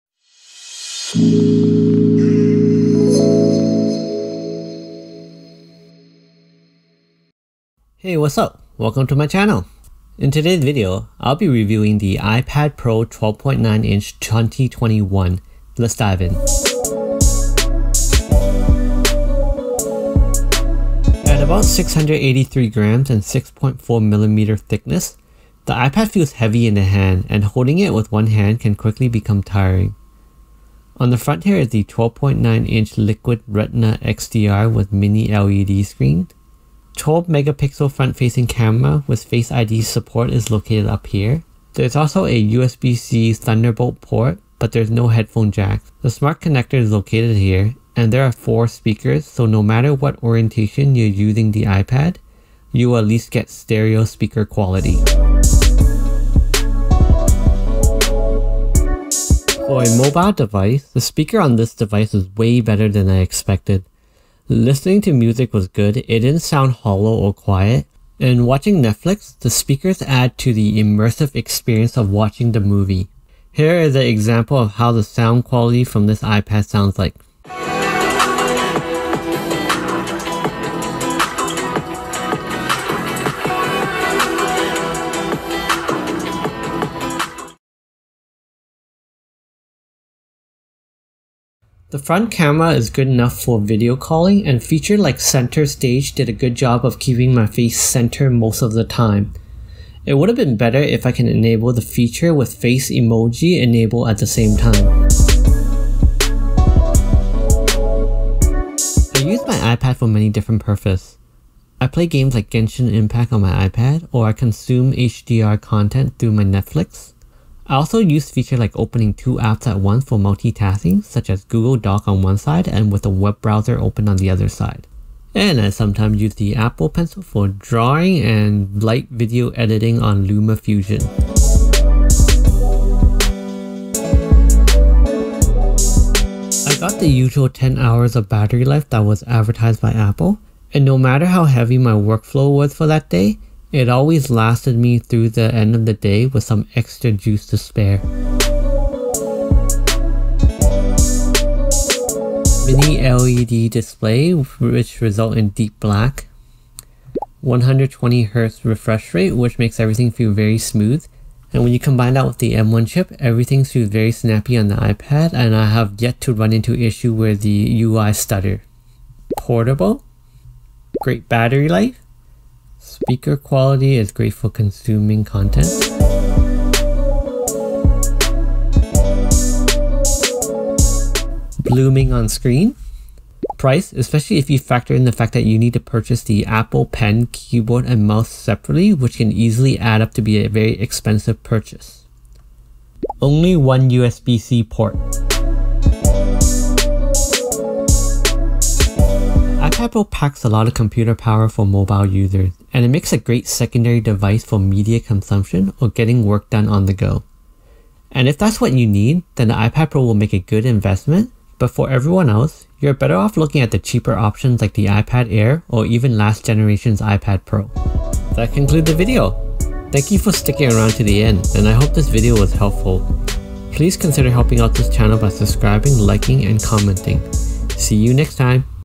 Should you purchase ipad pro m1?
This video is my review of the ipad pro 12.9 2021

Timecode
0:00 Intro
0:22 Initial Impression
0:55 Speakers
1:53 Center Stage
1:44 Usage
2:13 Battery Life
2:28 What i like
2:57 What i dislike
3:15 Should you purchase the ipad pro 2021?